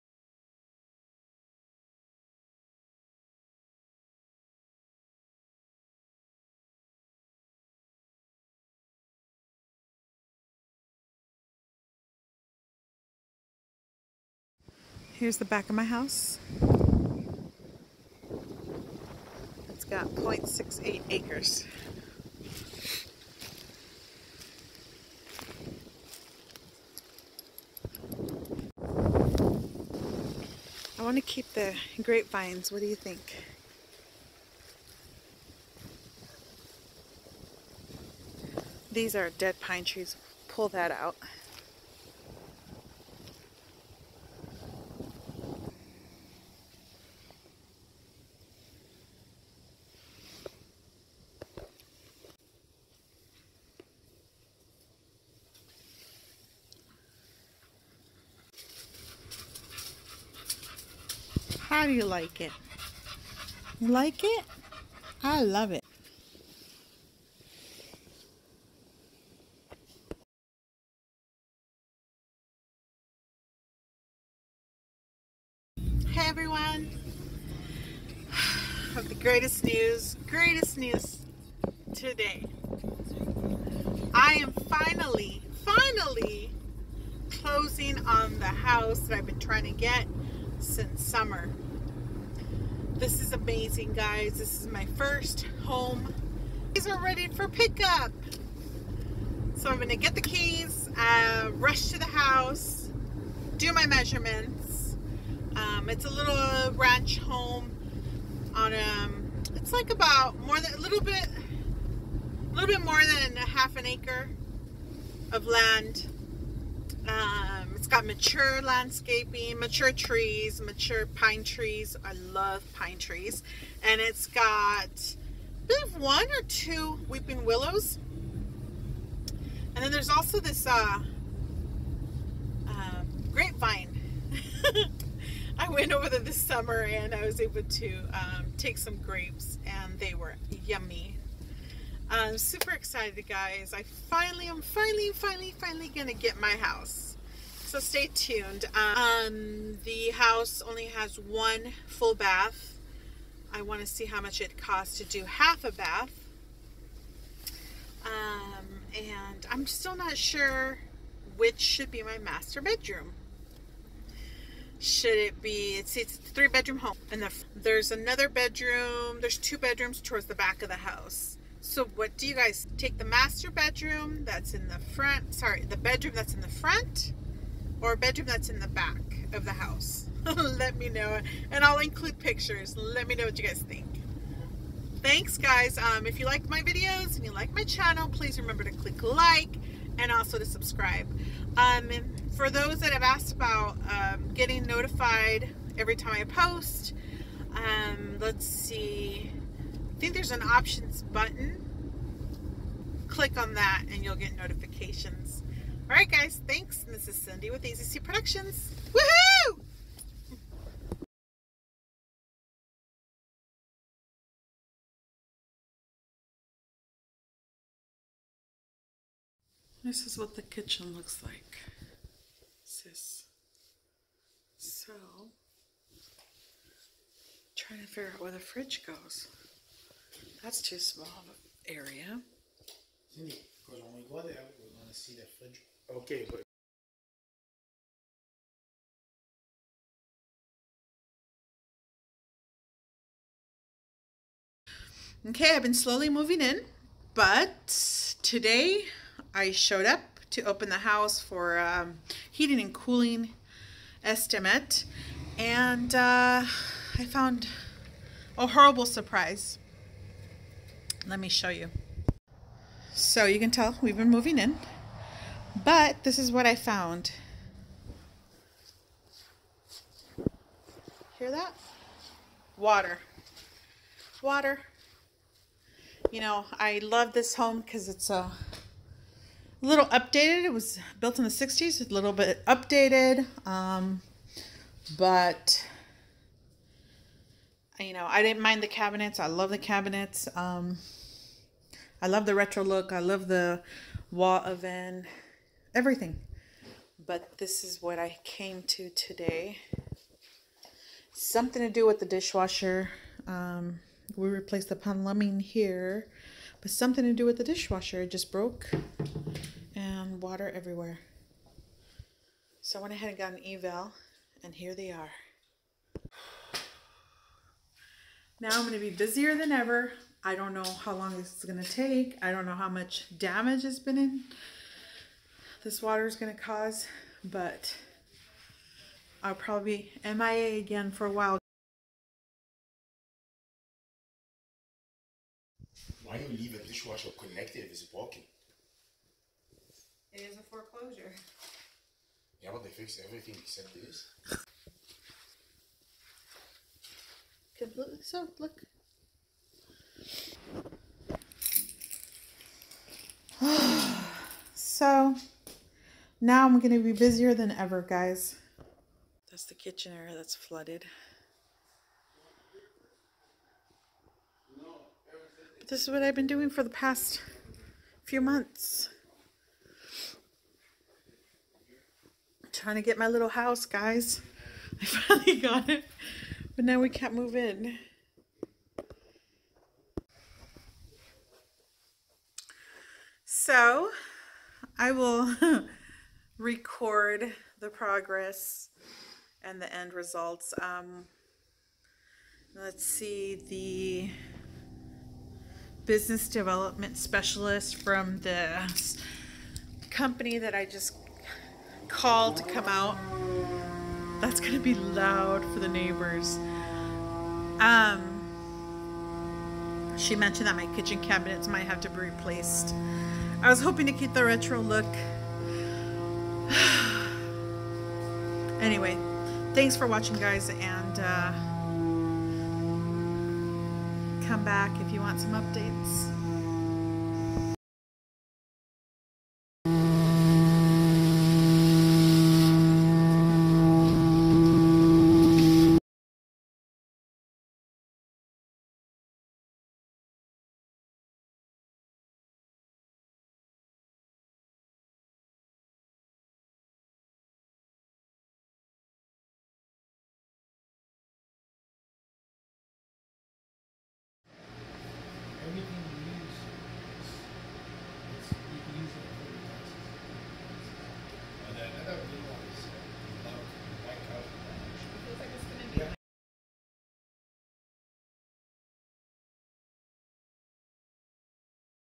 New Home but Then This Happens Blog
Great News with New Home Purchase but then watch to see what happens before I'm fully moved in. Bare in mind, I went through many ups and downs to finalize on this home. I am constantly told by my partner, "this house wasn't meant to be". But that isn't true, I see it as glass half full and once I'm done, she will fully embrace me...right?
Yes, you will hear that it is a foreclosure and there may be unseen repairs needed.
ugh..."Home Ownership Begans?!".
I'll document the progress.
Also, let me know, did you like this type of video?